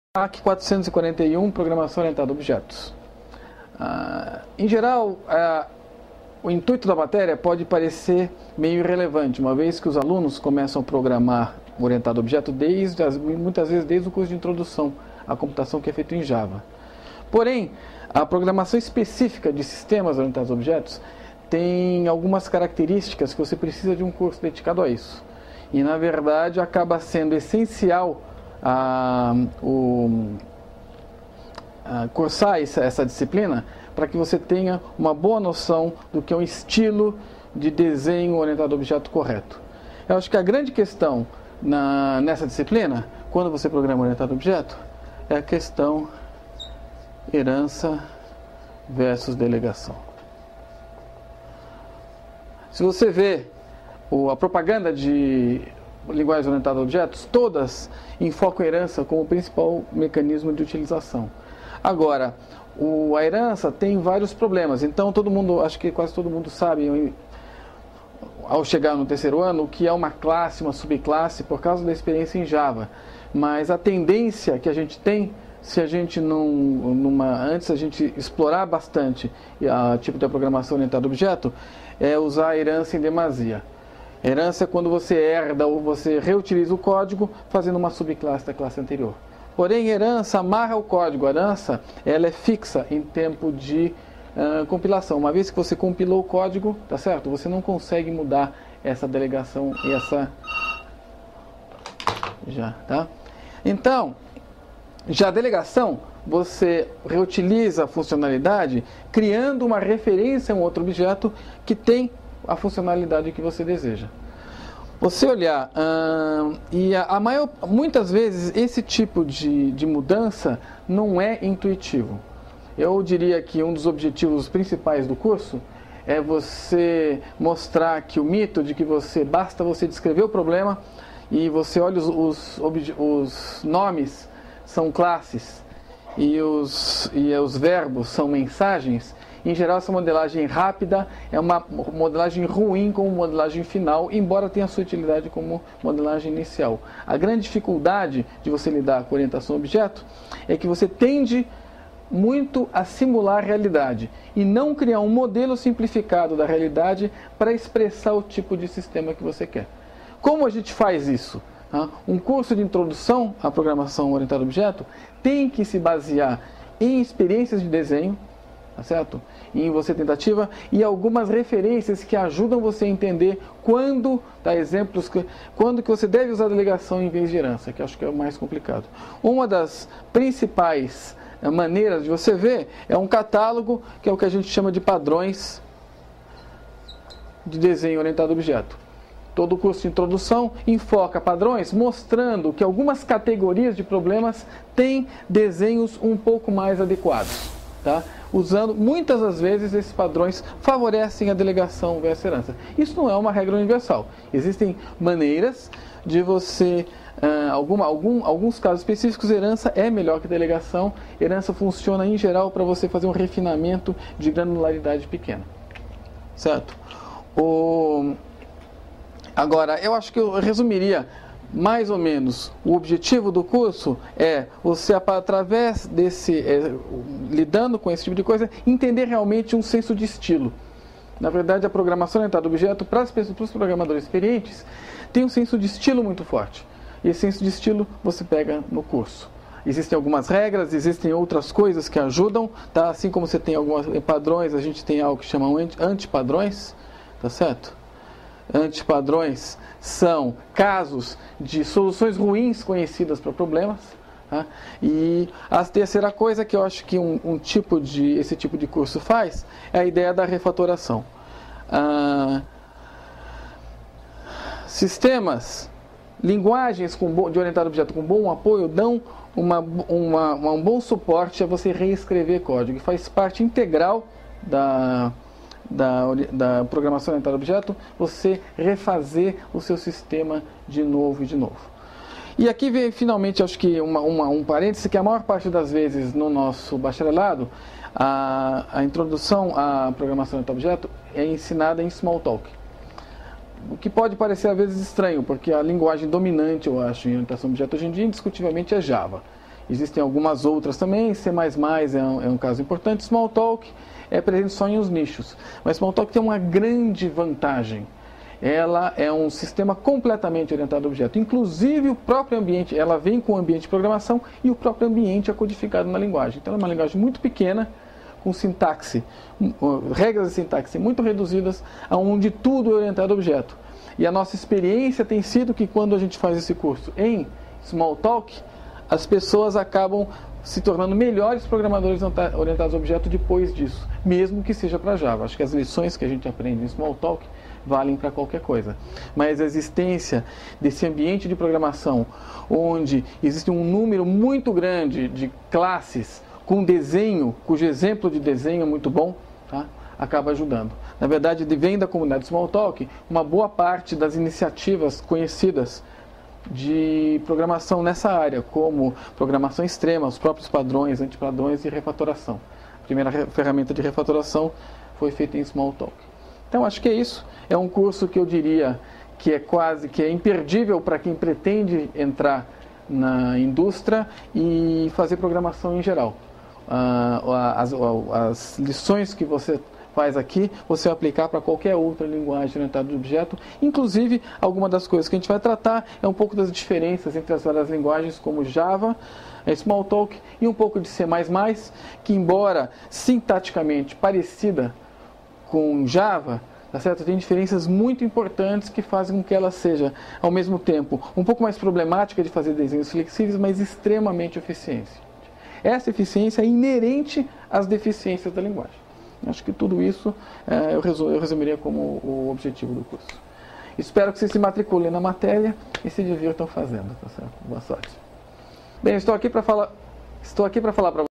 MAC0441 - Programação Orientada a Objetos
Descrição da disciplina "MAC0441 - Programação Orientada a Objetos" pelo professor Alan.